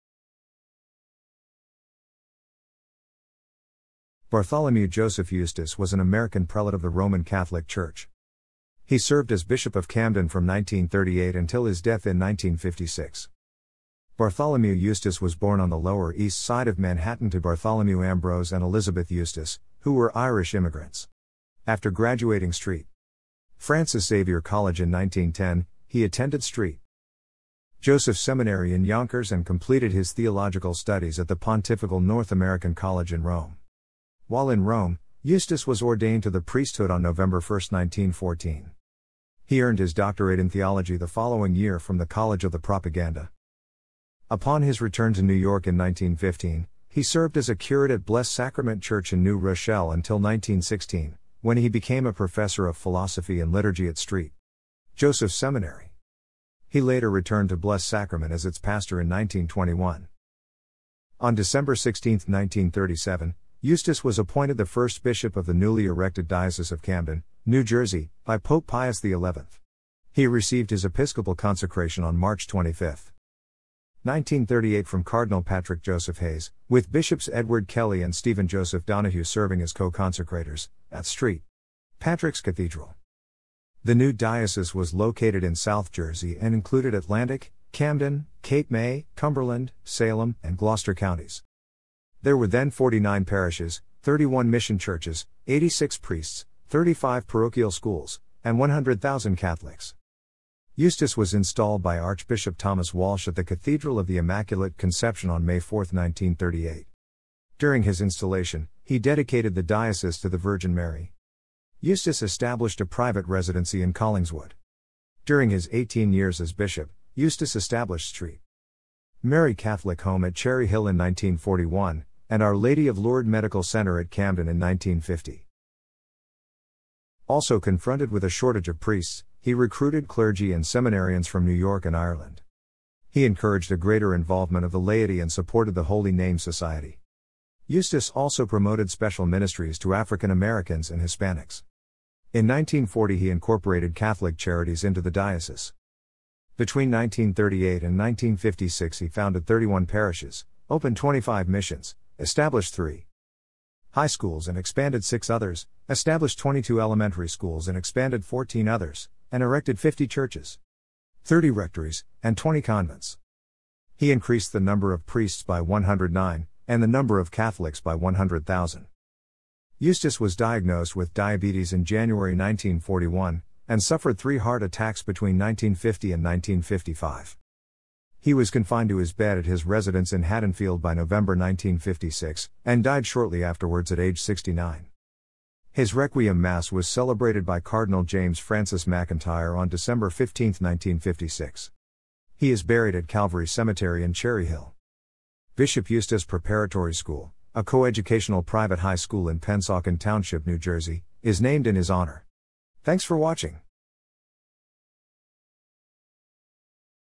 Bartholomew J. Eustace
People_from_Camden,_New_Jersey
People_from_Collingswood,_New_Jersey
People_from_Haddonfield,_New_Jersey
Saint_Joseph's_Seminary_(Dunwoodie)_alumni
20th-century_Roman_Catholic_bishops_in_the_United_States
Bartholomew Joseph Eustace （October 9, 1887—December 11, 1956） was an American prelate of the Roman Catholic Church.
He served as Bishop of Camden from 1938 until his death in 1956.
Bartholomew Eustace was born on the Lower East Side of Manhattan to Bartholomew Ambrose and Elizabeth （née Nolan） Eustace, who were Irish immigrants.
After graduating St.
Francis Xavier College in 1910, he attended St.
Joseph's Seminary in Yonkers and completed his theological studies at the Pontifical North American College in Rome.
While in Rome, Eustace was ordained to the priesthood on November 1, 1914.
He earned his doctorate in theology the following year from the College of the Propaganda.
Upon his return to New York in 1915, he served as a curate at Blessed Sacrament Church in New Rochelle until 1916, when he became a professor of philosophy and liturgy at St.
Joseph's Seminary.
He later returned to Blessed Sacrament as its pastor in 1921.
On December 16, 1937, Eustace was appointed the first Bishop of the newly erected Diocese of Camden, New Jersey, by Pope Pius XI. He received his episcopal consecration on March 25,
1938 from Cardinal Patrick Joseph Hayes, with Bishops Edward Kelly and Stephen Joseph Donahue serving as co-consecrators, at St.
Patrick's Cathedral.
The new diocese was located in South Jersey and included Atlantic, Camden, Cape May, Cumberland, Salem, and Gloucester Counties.
There were then 49 parishes, 31 mission churches, 86 priests, 35 parochial schools, and 100,000 Catholics.
Eustace was installed by Archbishop Thomas Walsh at th...